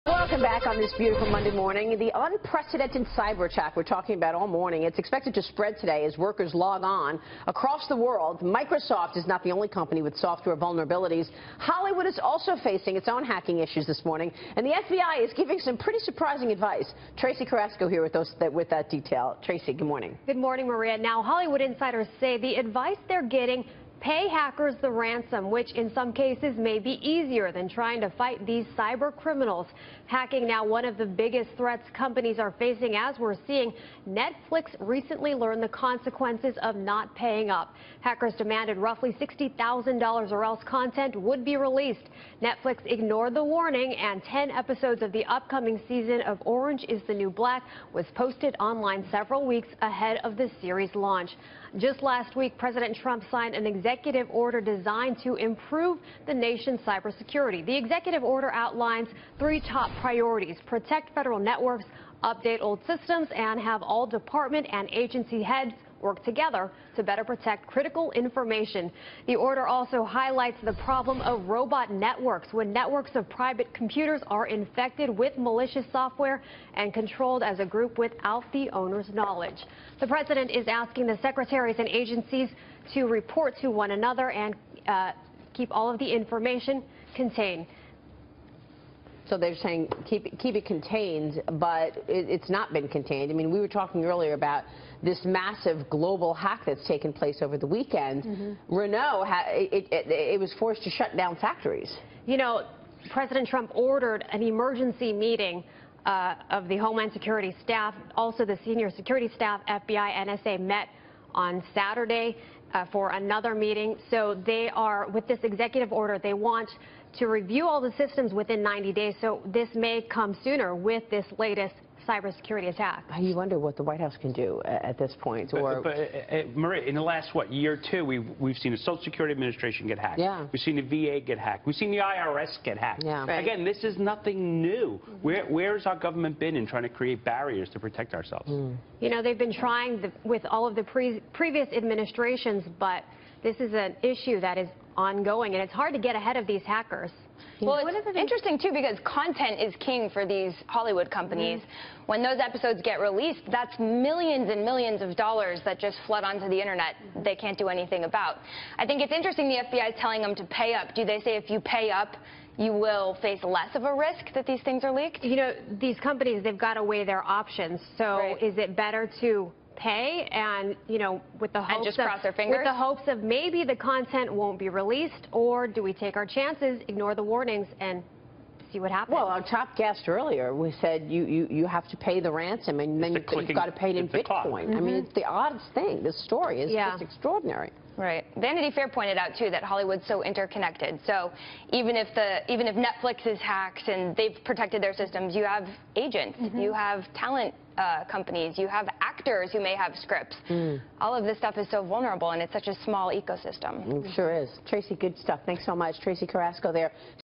FBI's advice to Hollywood on cyber attacks: Pay up
FBN's Tracee Carrasco on the impact of cyber attacks on Hollywood.